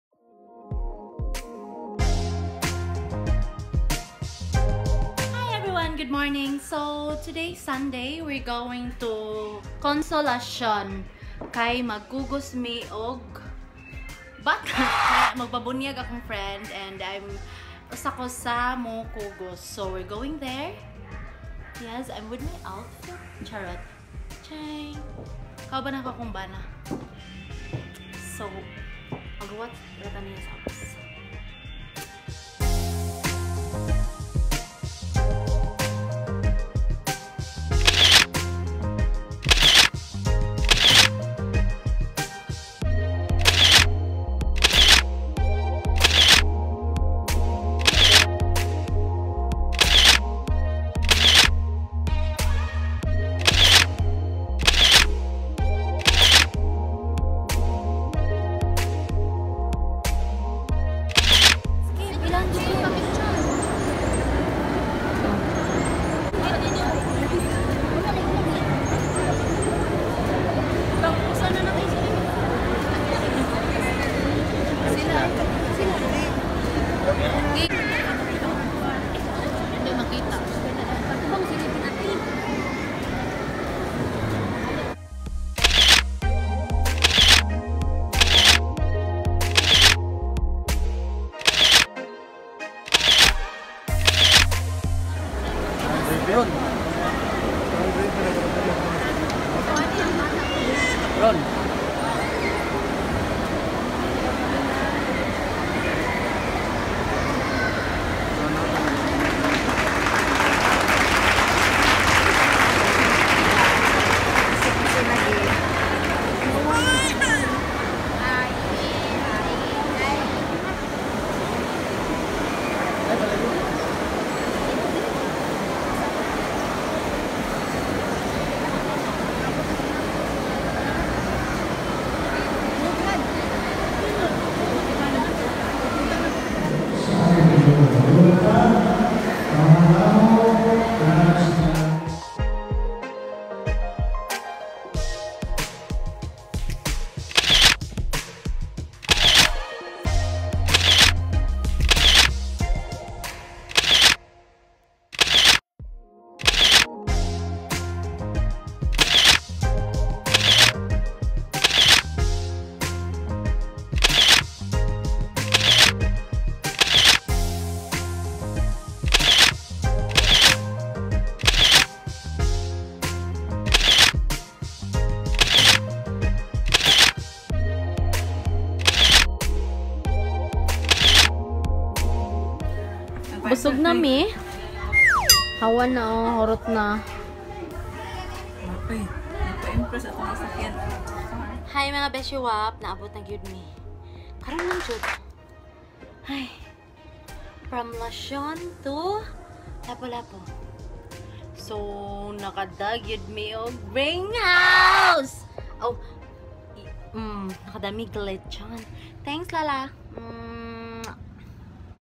Happy Christening John Nathan 😇|12-08-19|Congrats to the Parent's ❤️|Ninang ❤️
Happy Christening John Nathan..just a quick video for all of you beshh,,pasensya ug wa kayu na focus sa bb ang vid kai lisud kau d pwd muadtu sa tubangan sa bata naa rjud mi sa kilid ana...
thanks to my hubby. ❤️